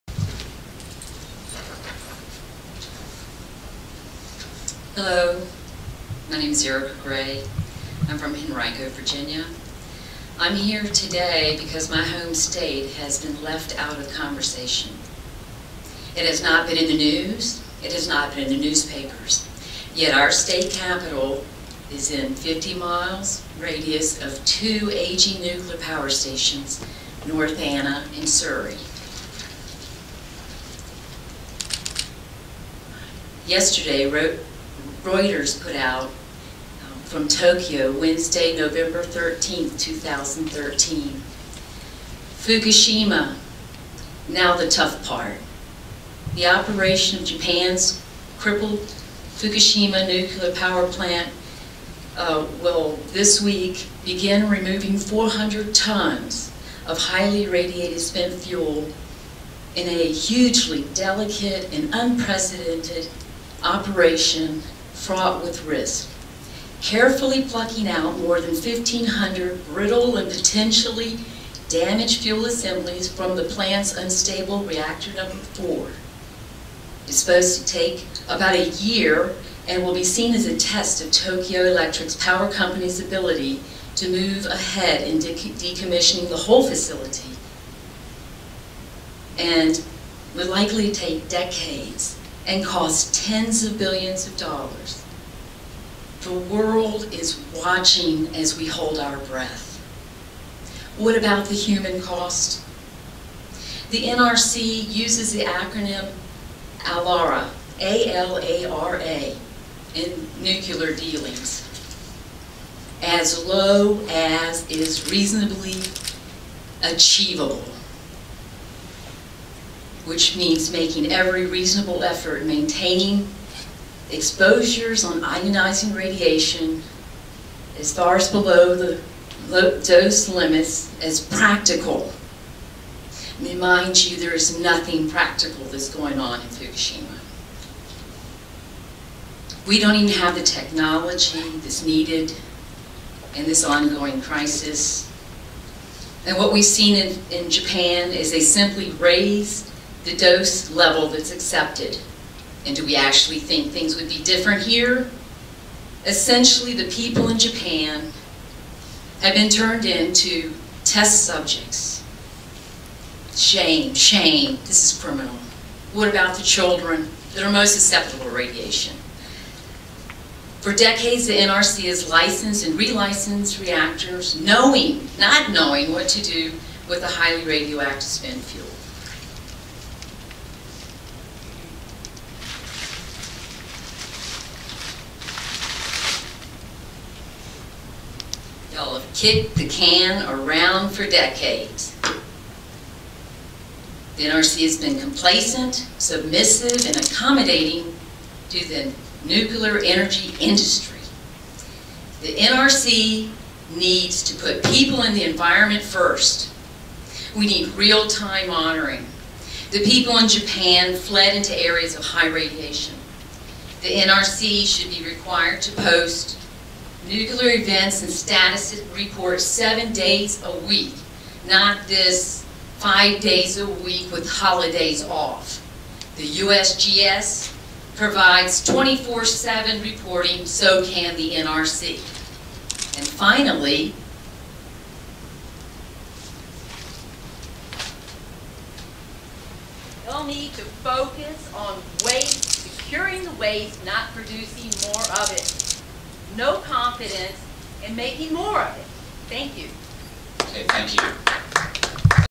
Erica Gray Commenting Nuclear Waste Confidence Meeting Thu.Nov.14 2013
Nrc.Hq Rockville Md. Thu.Nov. 14 2013